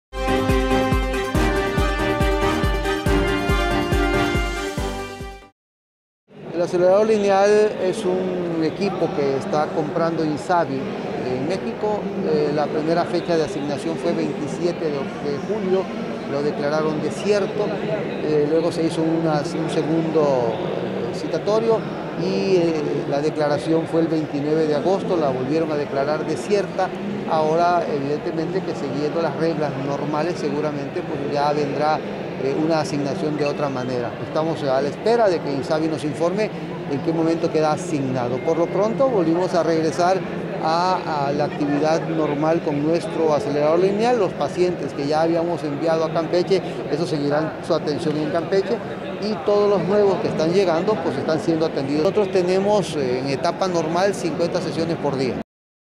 Acelerador lineal será adquirido por adjudicación directa
NoticiasTVT🟣 La adquisición del 'acelerador lineal' del Hospital de Alta Especialidad 'Juan Graham Casasús' será por adjudicación directa, pues en dos ocasiones se ha declarado 'desierta' la licitación, indica su director Víctor Narváez Osorio.